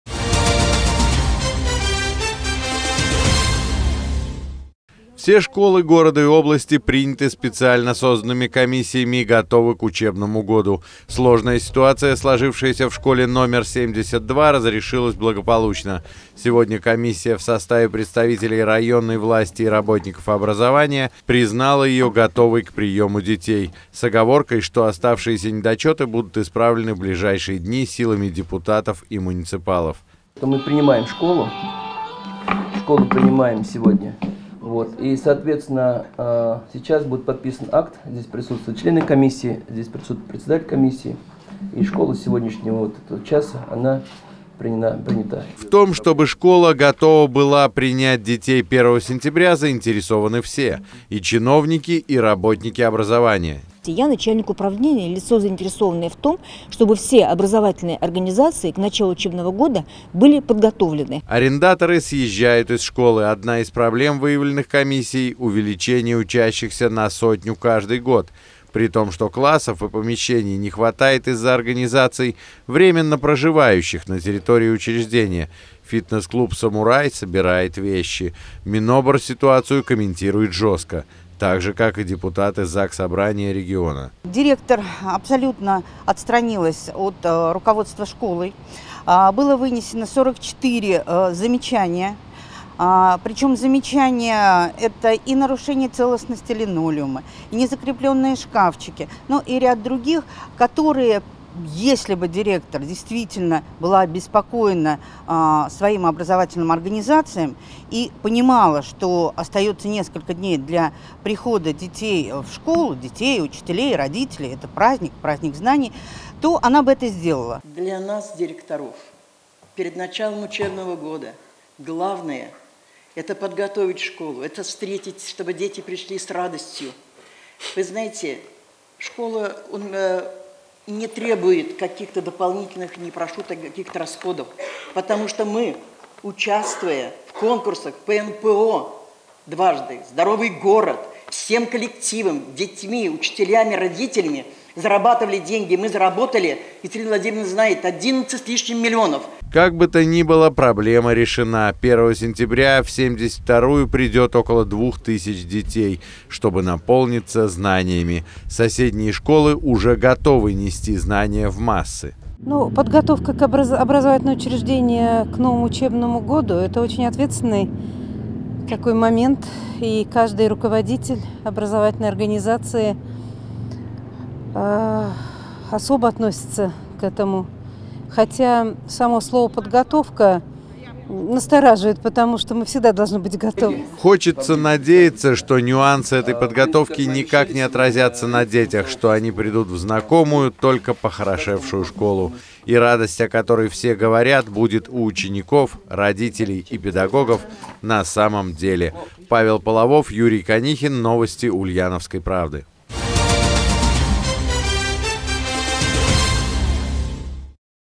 Минобр, администрация, общественники, педагоги за! 72 школу приняли к новому учебному году
Все школы города и области приняты специально созданными комиссиями и готовы к учебному году. Сложная ситуация, сложившаяся в школе №72, разрешилась благополучно.